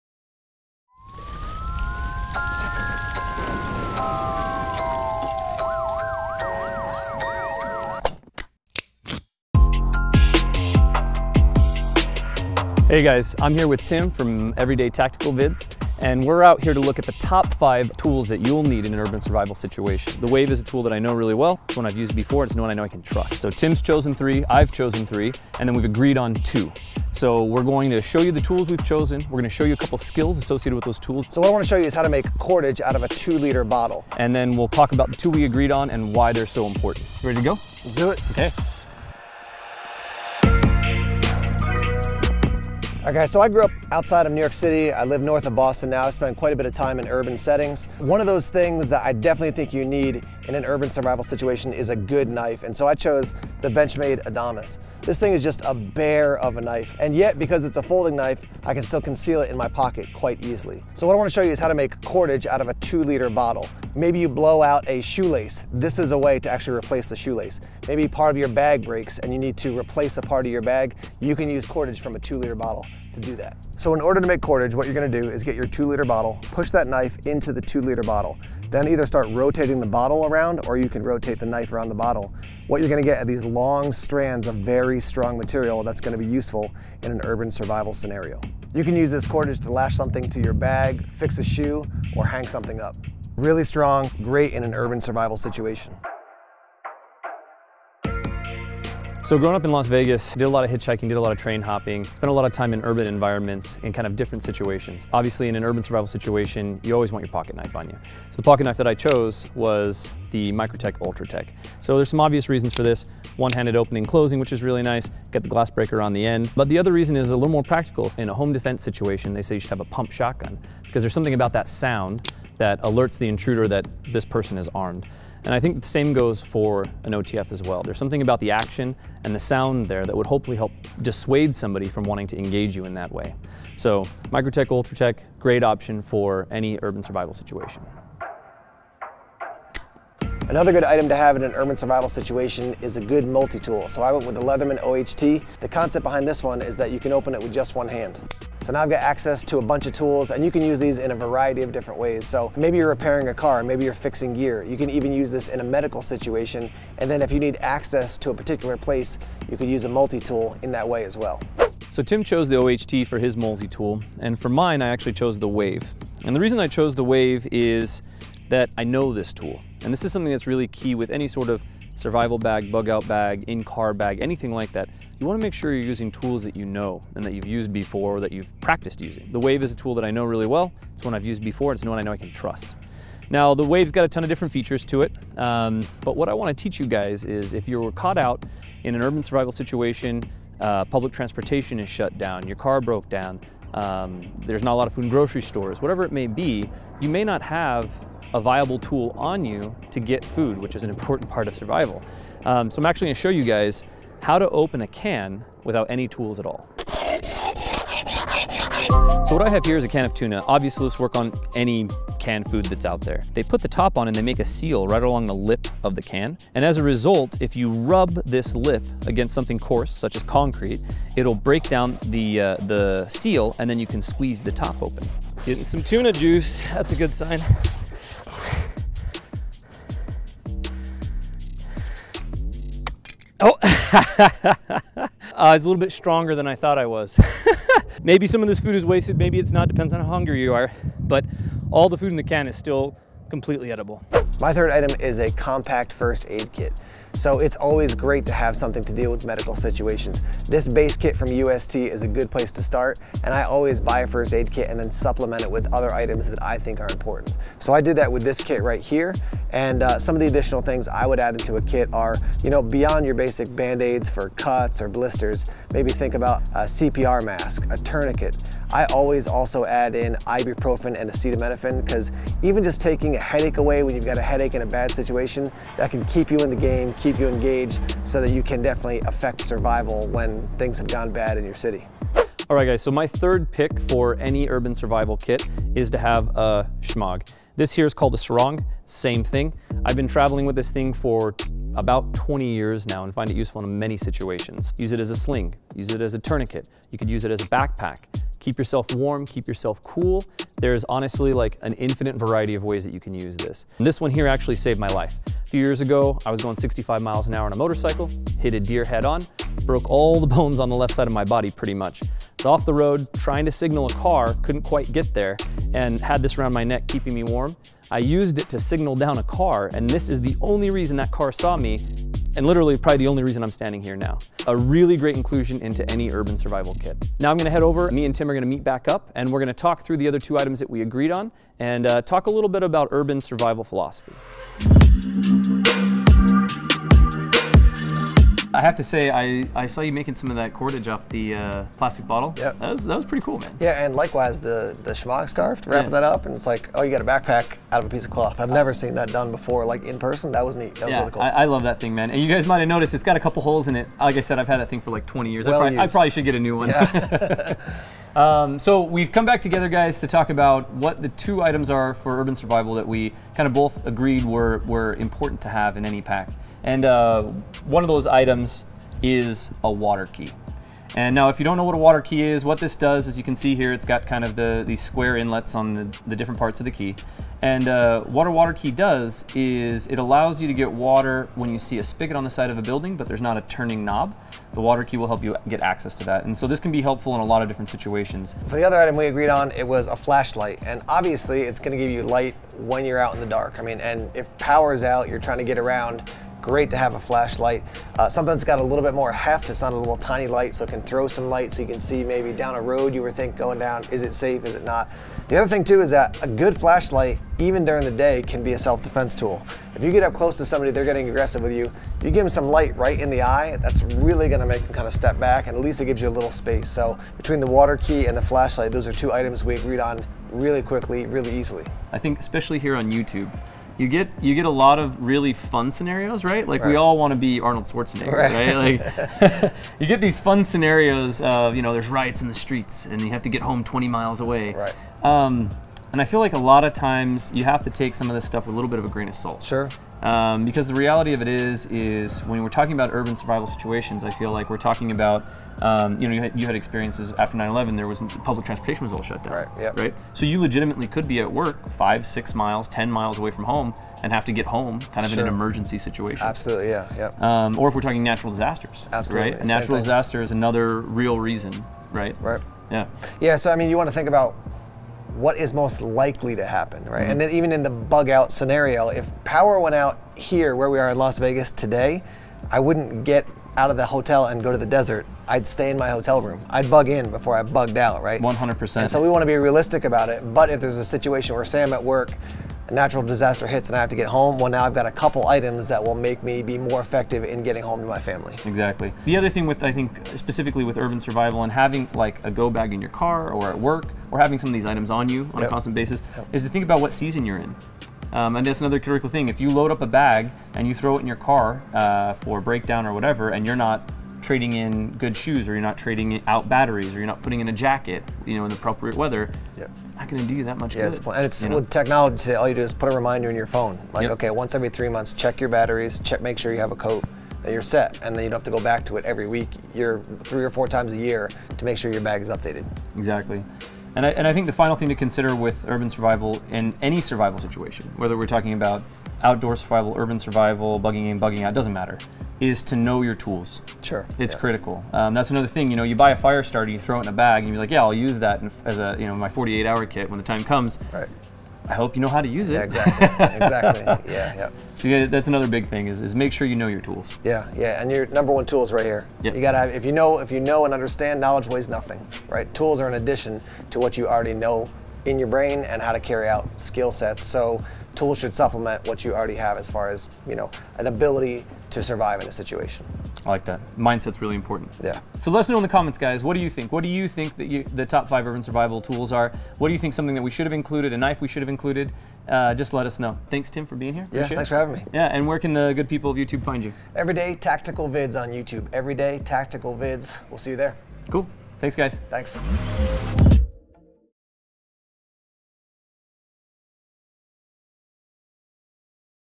Top 5 Tools for Urban Survival
Zac meets up with Tim from EverdayTacticalVids to talk about the top 5 tools for urban survival.

Never Miss A New Knife!